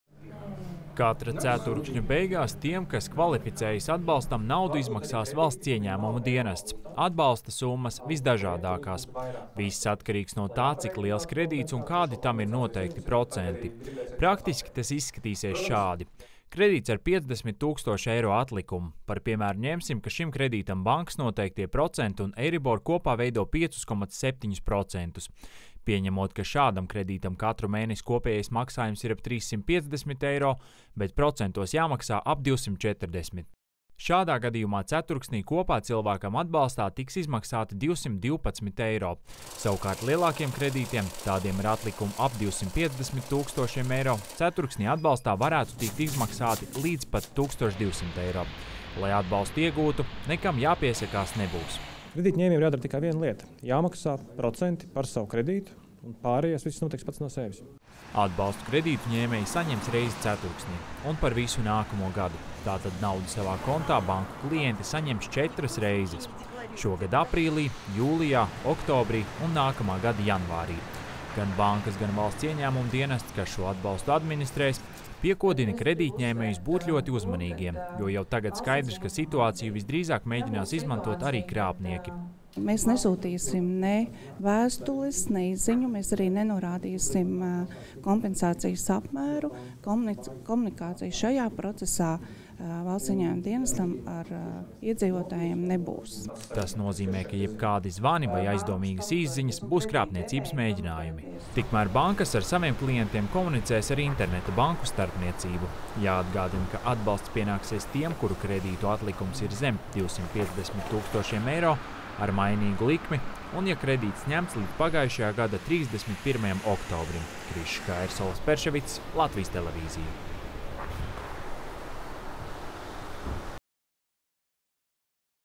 Hipotekāro kredītu atbalstam atsevišķi pieteikties nevajadzēs
Pērn Saeimā atbalsts hipotekārajiem kredītņēmējiem bija viens no plašākajiem diskusiju tematiem. Sākumā viena versija, tad cita. Beigās deputāti vienojās noteikt kredīt- ņēmējiem atbalstu 30 procentu apmērā no visiem procentu maksājumiem, bet ne vairāk kā par diviem procentpunktiem.